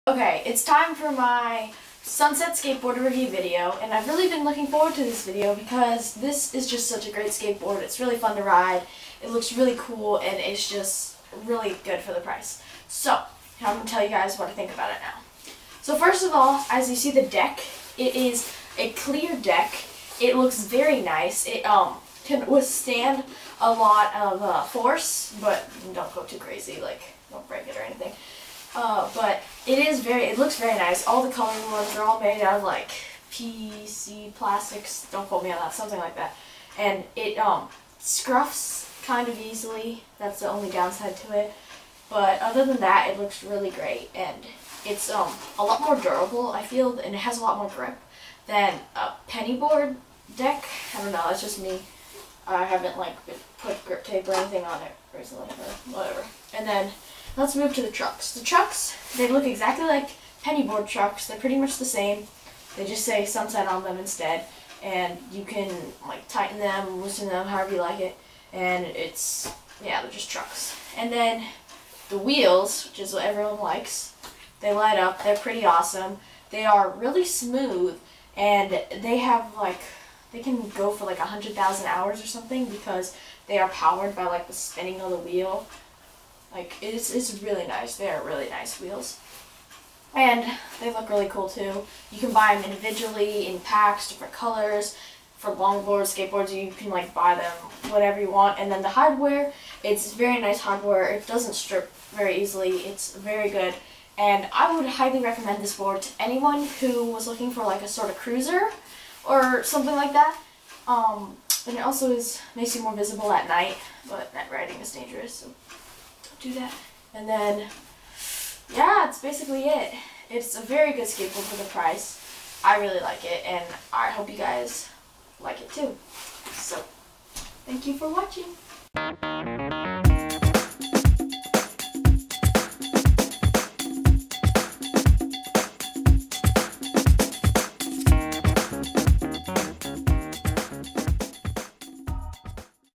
Sunset Skateboard Review
A quick review of my new Sunset Skateboard and some night-riding at the end.